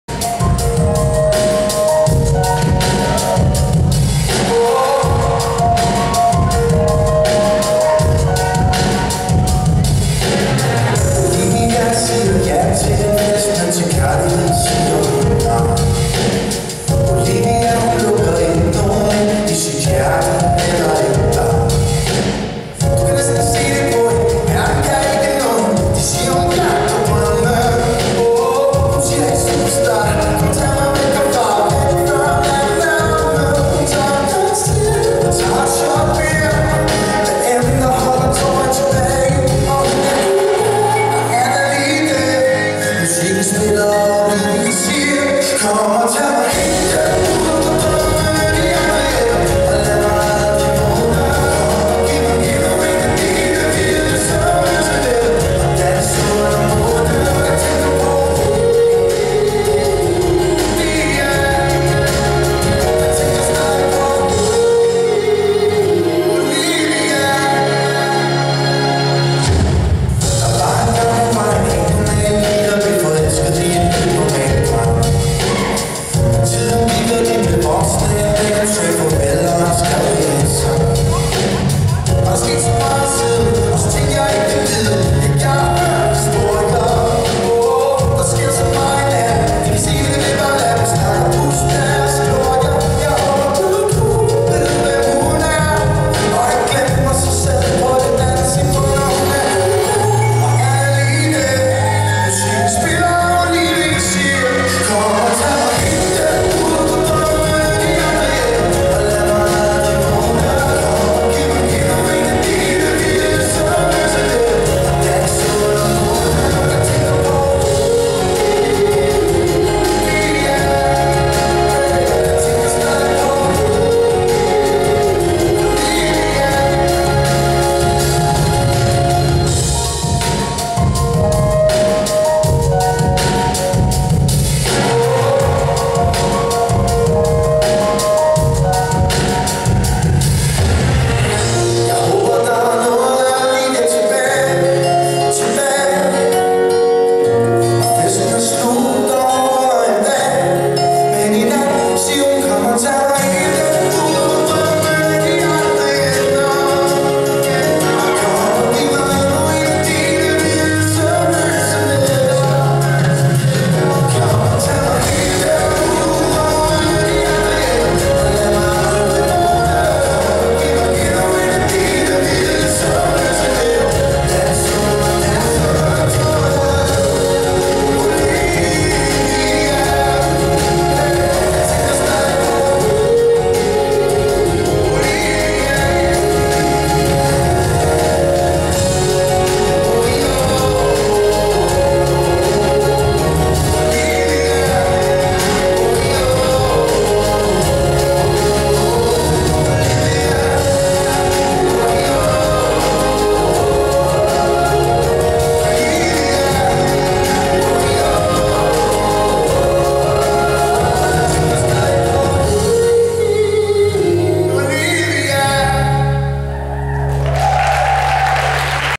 Borup Zumba - Olivia v/Diana
Mega nytårsevent i Multihallen i Borup med Diana & Hanita
Lyd/lys: DISKODASKO.COM v/Carsten Jensen